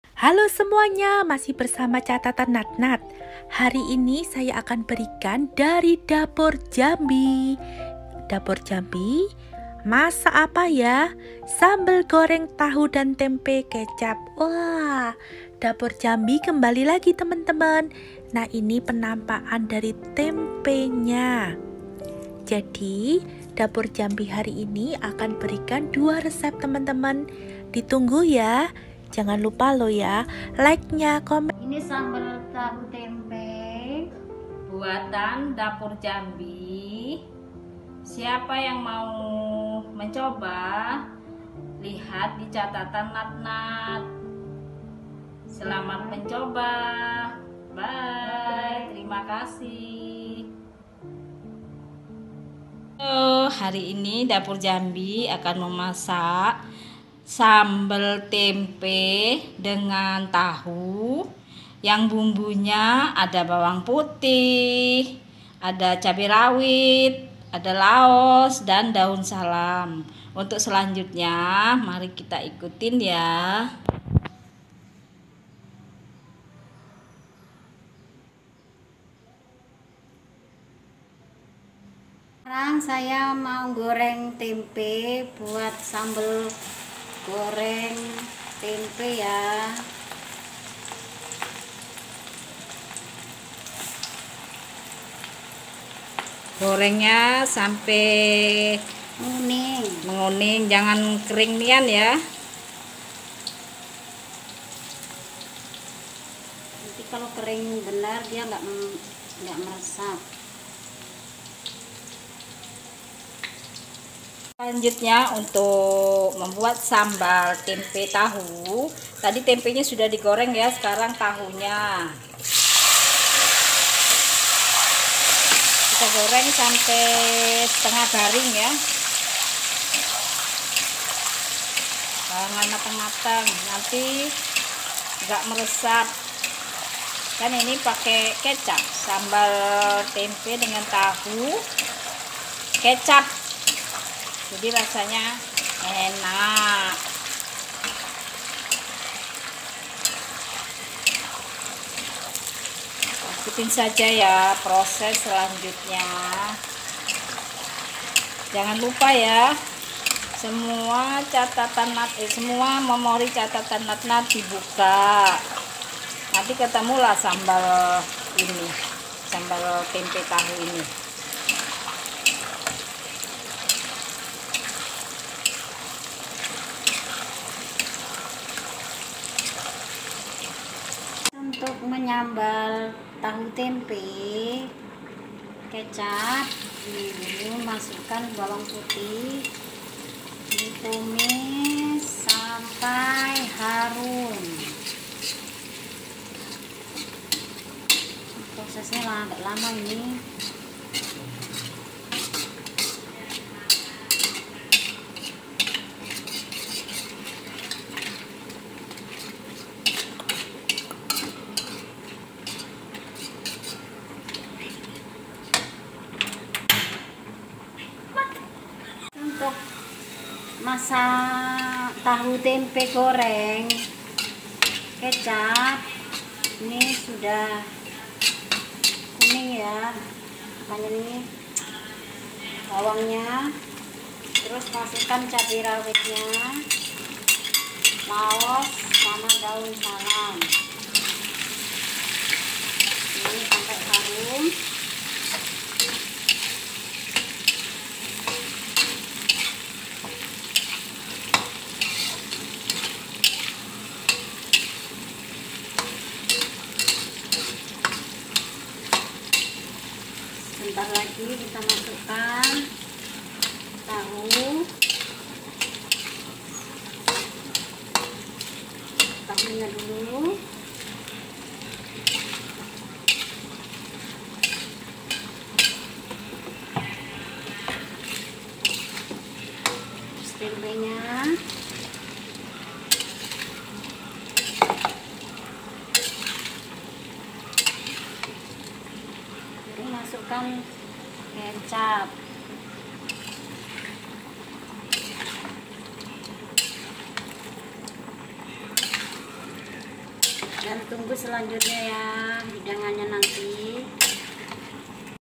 Sambal Goreng Tahu & Tempe Kecap di Dapur Jambi
Selamat siang semuanya
Hari ini Dapur Catatan NatNat ke 2
Dapur Jambi
Masak Sambal Goreng Tahu & Tempe Kecap
Langsung di tonton ya 🌻
Terima kasih 💕